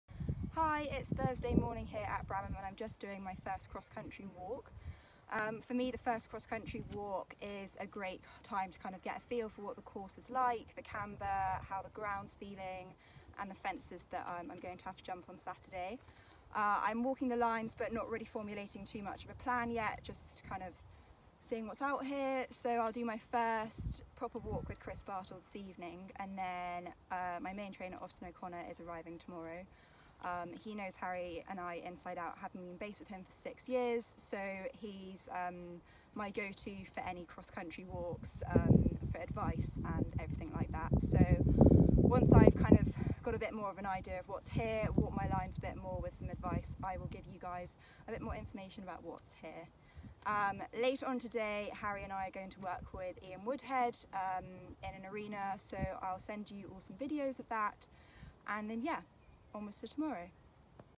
India Wishart & The Masters Harry at Bramham CCI-Lu25 4* - Walking the XC course
India Wishart & The Masters Harry at Bramham CCI-Lu25 4*
Catch up with India walking the big XC course at Bramham! Head 2thebarn.com for more!